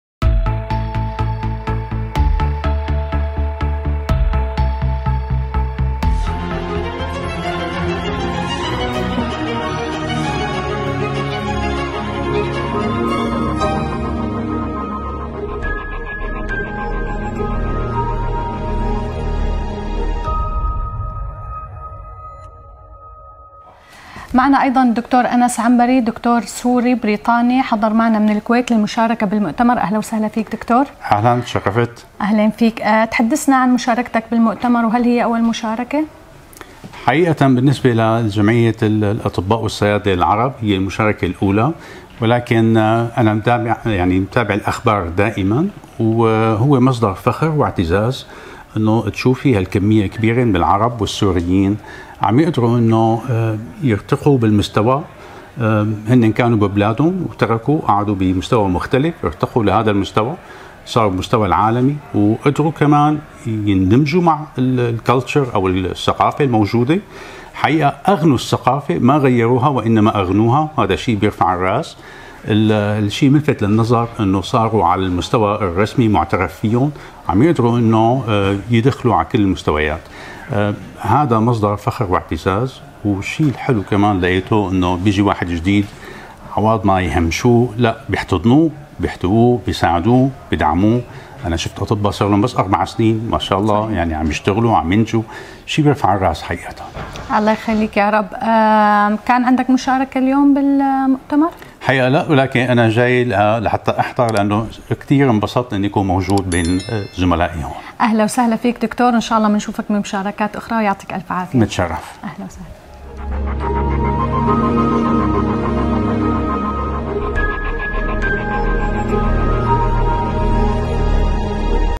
مؤتمر الاطباء العرب ال ٢٩ في فيينا وقفة مع د. أنس عنبري استشاري امراض العيون
مقتطفات حوارية من وقائع مؤتمر الاطباء العرب في النمسا 2023
البرفيسور أنس عنبري استشاري امراض عيون وهو طبيب سوري وله حضوره في المحافل الدولية الطبية
وقفة من مؤتمر الاطباء العرب في النمسا مع د. أنس عنبري استشاري امراض العيون ومعالجتها